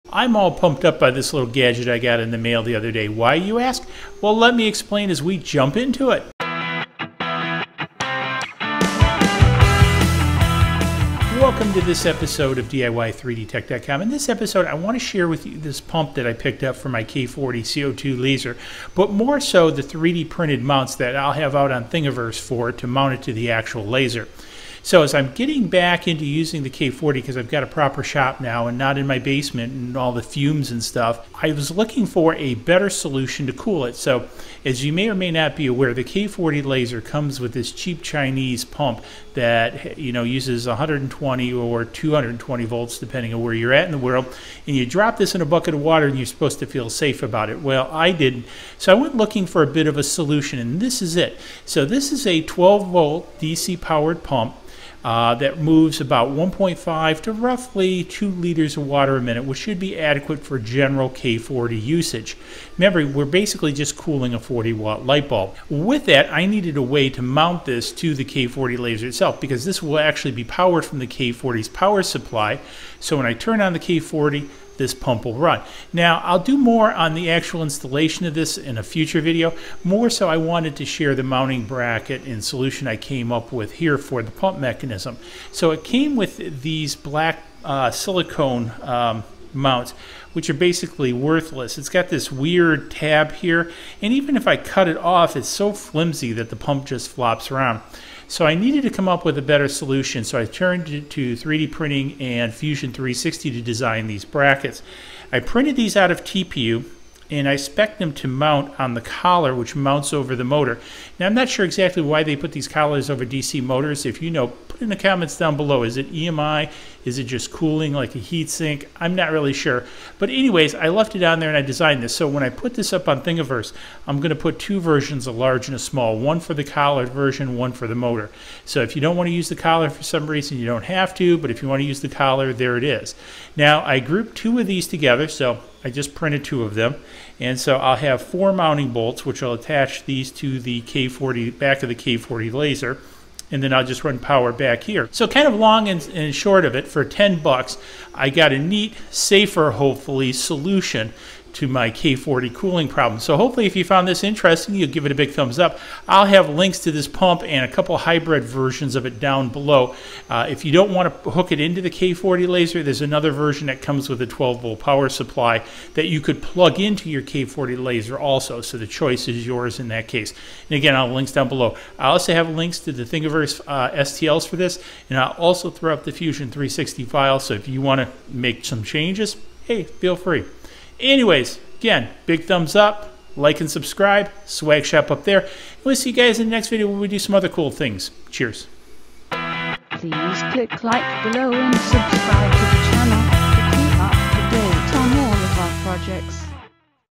eBay K40 CO2 Laser - New Safer Cooling Solution!
In this episode, we will be looking at the mounting brackets we designed to attach a 12 volt (DC) pump to our eBay K40 CO2 Laser as I felt uncomfortable droping a Chinese pump with 120 volts in a bucket of water.  So this seems to make more sense as it moves 1.8 liters per/min and planing to use power from the laser from itself to drive the pump.  There are a couple versions:

1. as this version comes with its own 12volt power supply so you don' have t open the laser and do any wiring.

2. as this version is half the price of the first, yet you will need to come up with your own power solution.

to what we are up too as we always have something cooking in the shop!